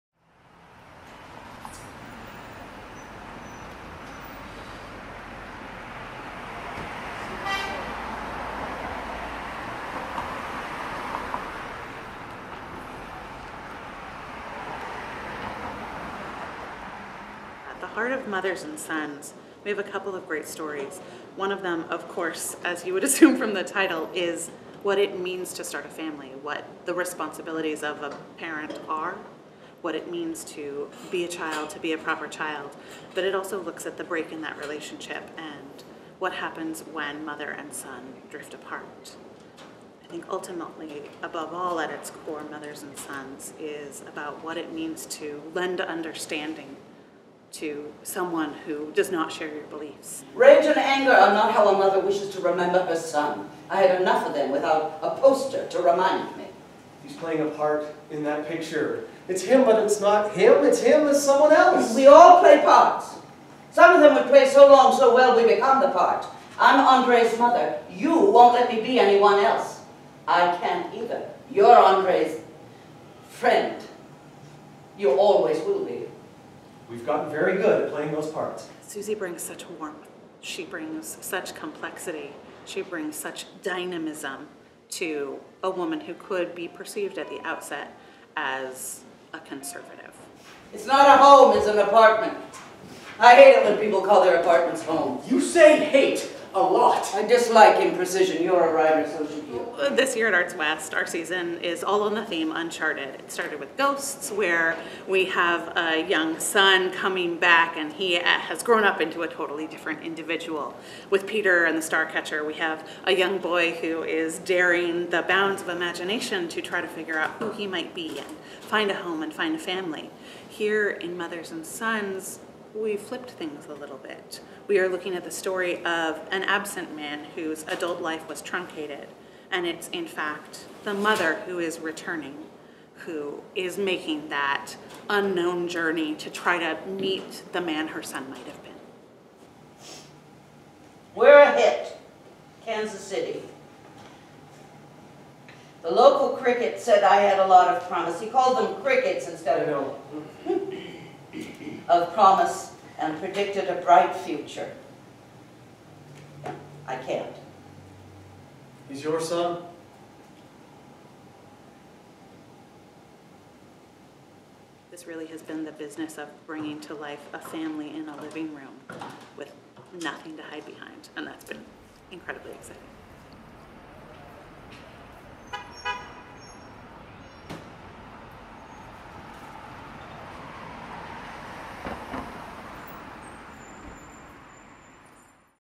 MOTHERS AND SONS Trailer
Get a glimpse into Terrence McNally's Tony Award-nominated play MOTHERS AND SONS, the new stage production at ArtsWest. By turns smartly funny and powerfully resonant, this latest play from acclaimed dramatist Terrence McNally (Love! Valour! Compassion!, Corpus Christi, Master Class) portrays a woman who pays an unexpected visit to the New York apartment of her late son’s partner, who is now married to another man and has a young son. Challenged to face how society has changed around her, generations collide as she revisits the past and begins to see the life her son might have led.

Directed by ArtsWest Producing Associate Makaela Pollock and starring Suzy Hunt, Evan Whitfield, Jason Sanford and Isaac Spence. Photos by John McLellan. Tickets and more information at artswest.org